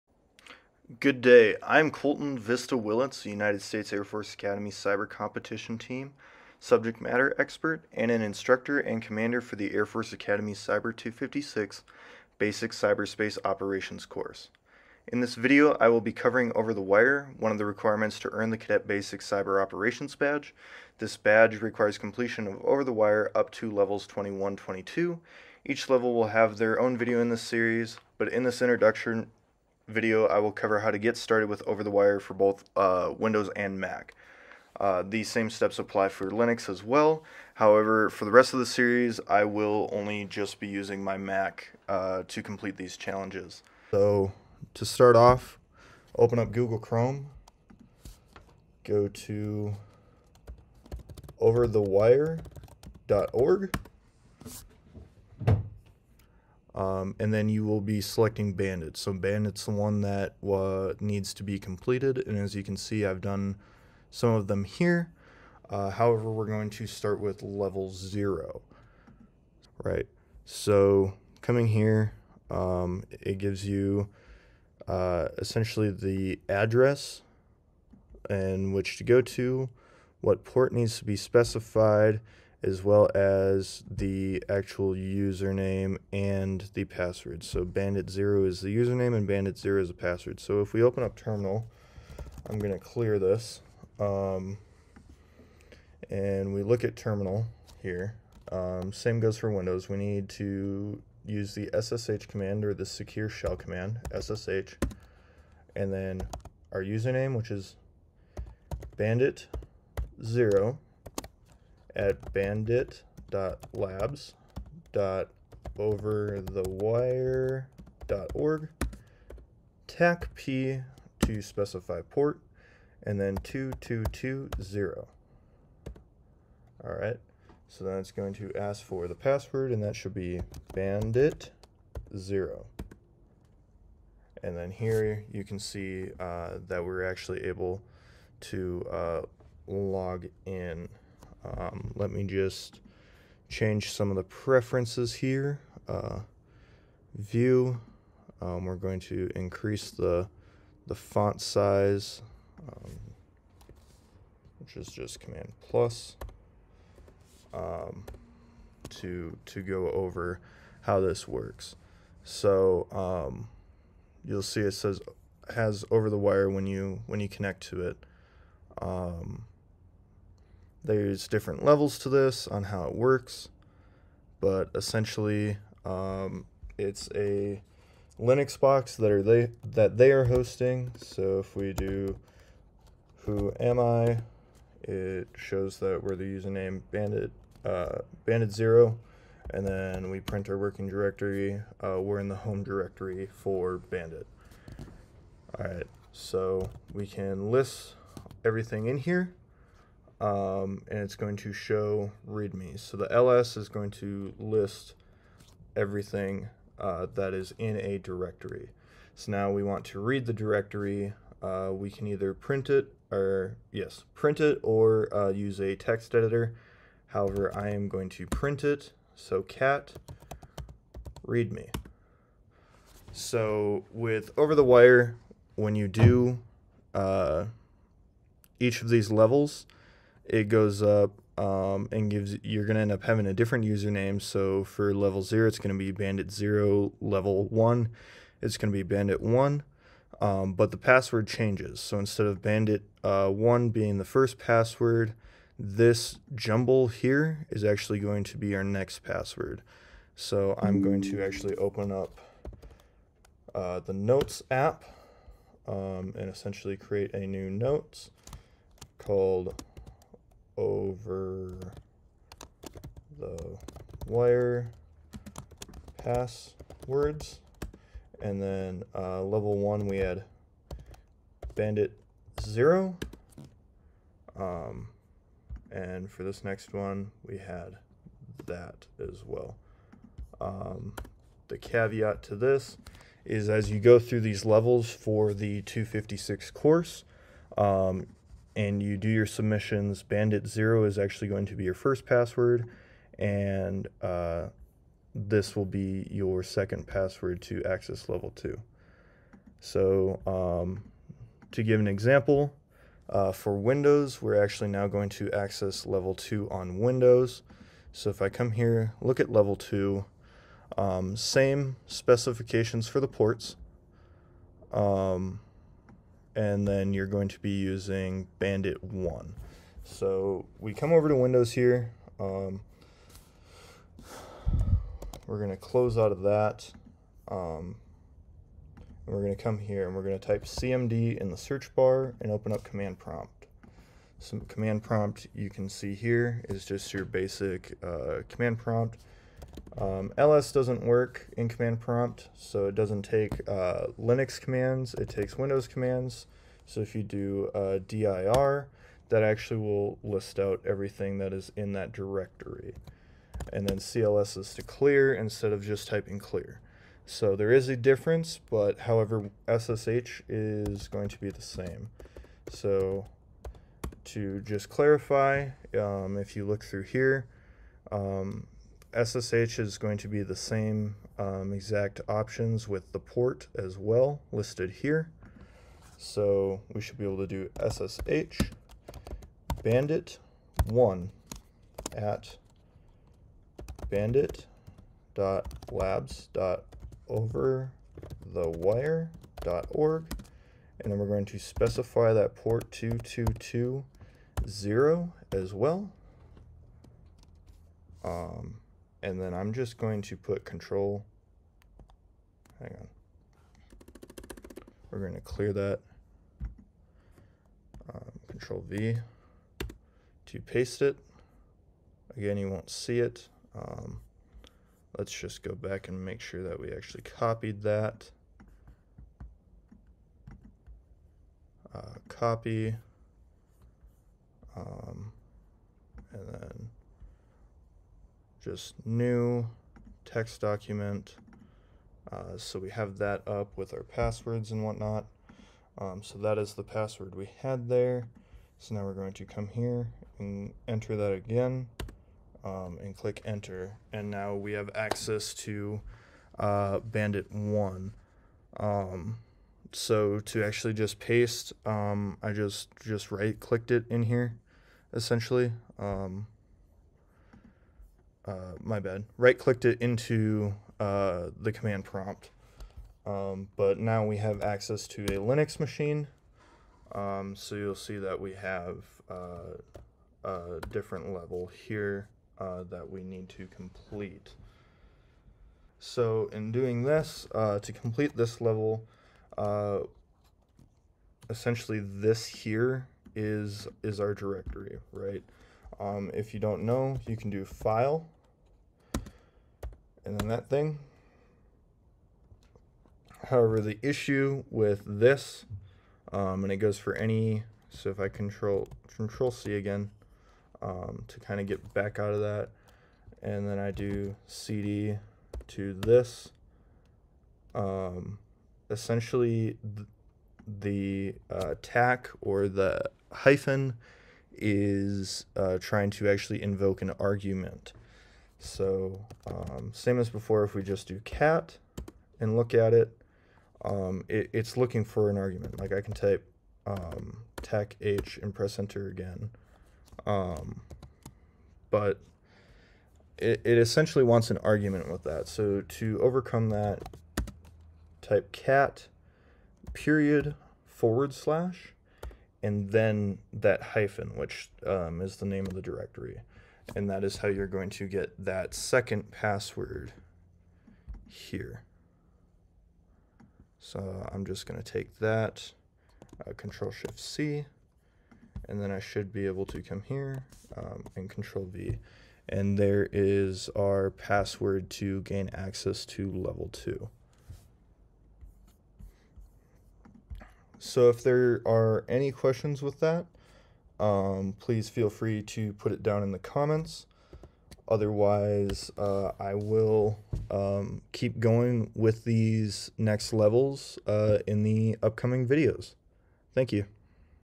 OverTheWire Bandit Setup and Introduction Level 0 through 01-02
This video is an introduction to OverTheWire Bandit for USAFA's Cyber 256 Course. It covers how to access Bandit through Mac and Windows, and gets students started on the OTW process through levels 01-02.